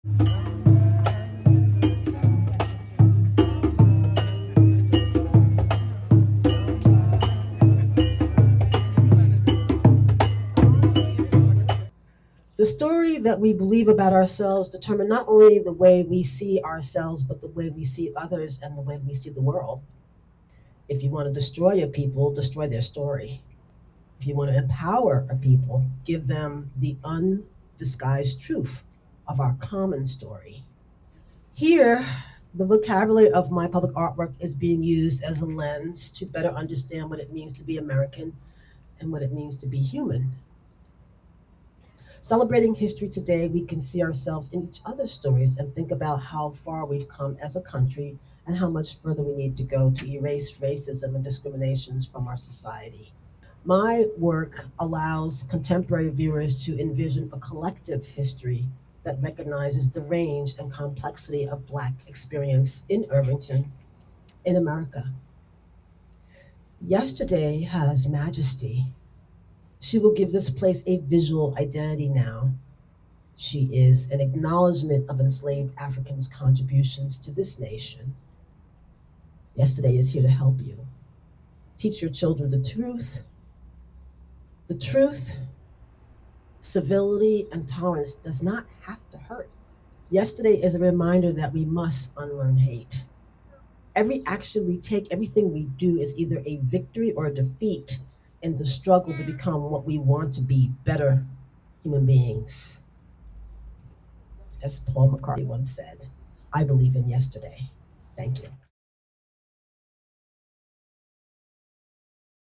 07 - Remarks by artist Vinny Bagwell (excerpt)
Location: Main Street School (near Madam CJ Walker Plaza), Irvington, NY
Sculpture: Yesterday
Artist: Vinny Bagwell

Commemorating Enslaved Africans in the Village of Irvington, New York.

From the 1600s through the abolition of slavery in New York State in 1827, the prosperity of the farms that eventually became the Village of Irvington was inextricably tied to the forced labor of enslaved African men, women, and children who lived and toiled here. Fourteen names emerge in the archival records:
Zusanna Deckse, Hannah, Betty, Dick, David, Teem, Brebay, David, Bill, Jack, Bette, Bet, Caesar, and Hannah. The personal identities of the rest of lrvington's enslaved Africans have been lost forever, appearing as mere numbers in census records, wills, estate inventories, and bills of sales.
On this site, we honor the legacy of enslaved Africans, and reaffirm their humanity which was stripped from them in their time. These people deserve to be recognized, discussed, and remembered. May this reckoning help heal the wounds of the past and sow the seeds for everlasting peace for all of the people of our village, this nation, and the world.
--The Commemorate Committee

Video by: Kate Hanna, Michael Hanna